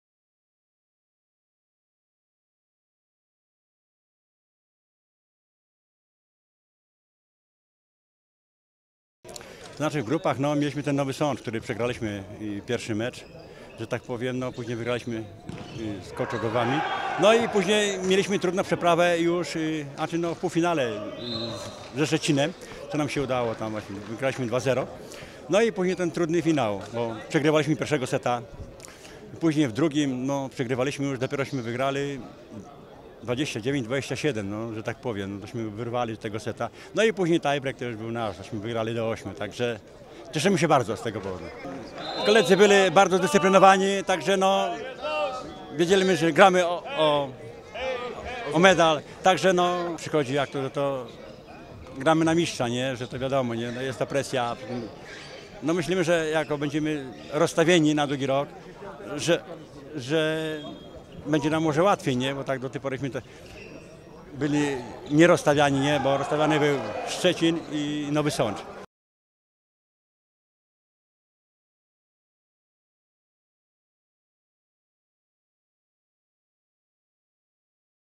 XXII ORLEN MPO: poznajmy się – Krzysztof Walczak
XXII ORLEN Mistrzostwa Polski Old Boyów w piłce siatkowej im. Wojciecha Zielińskiego rozgrywane były w Kędzierzynie-Koźlu. Rywalizowało w nich kilkuset zawodników w sześciu kategoriach wiekowych, od najmłodszych 35 plus, po najstarszych 60 plus.